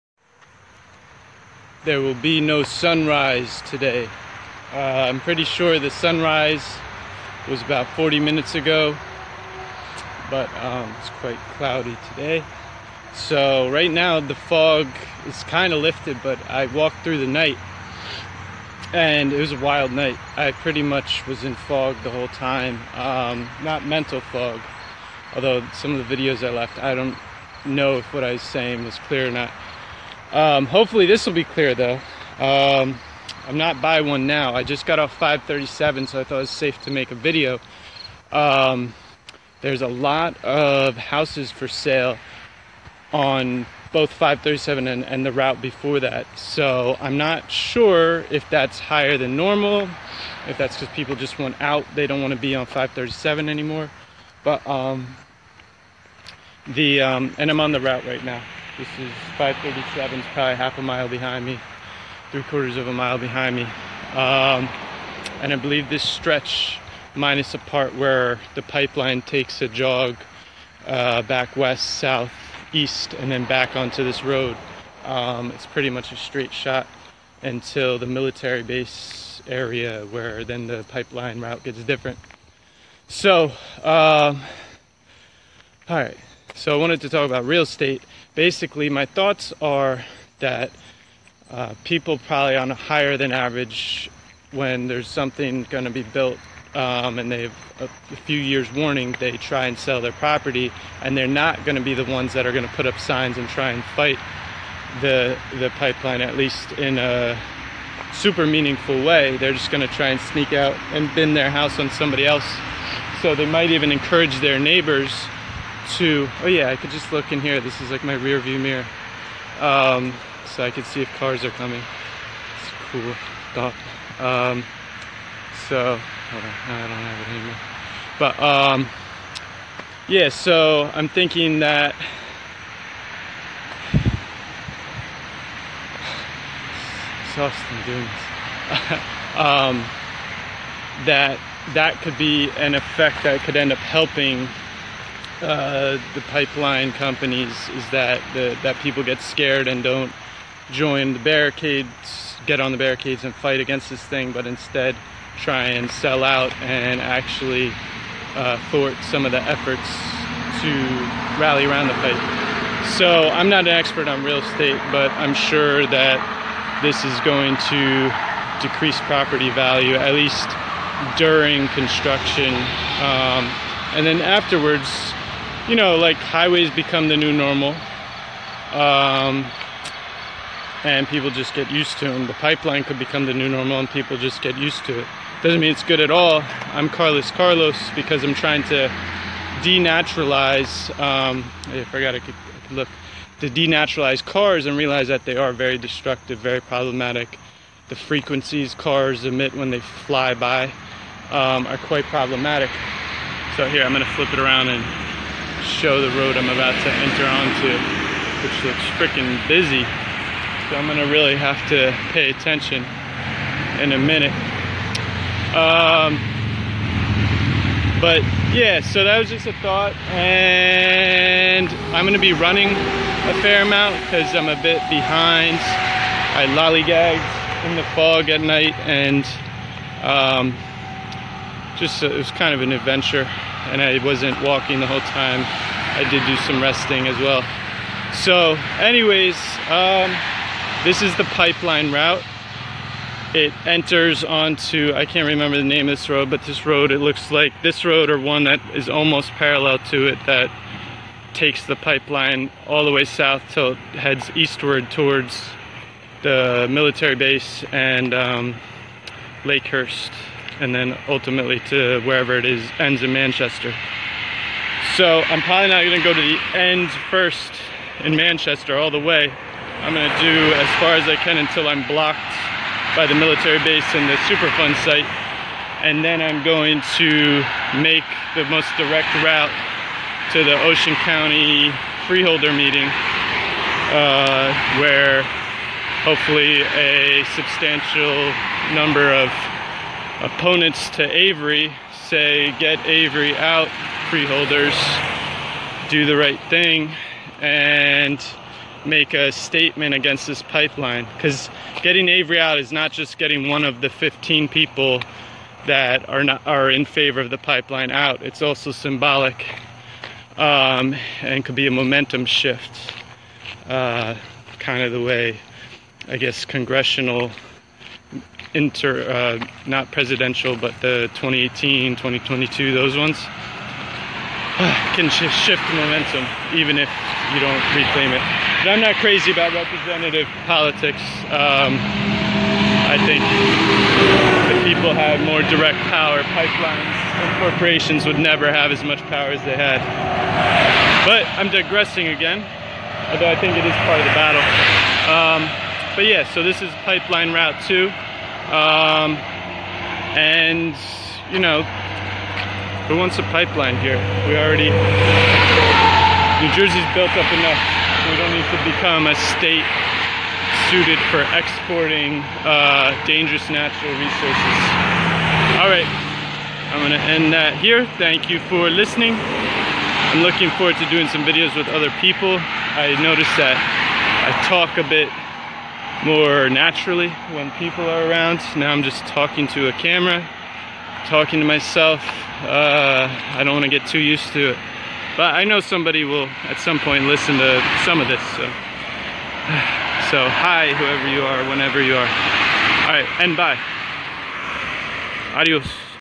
Carless Karlos - SRL pipeline and real estate, just after sunrise
Are people selling out of the area because of the specter of the pipeline?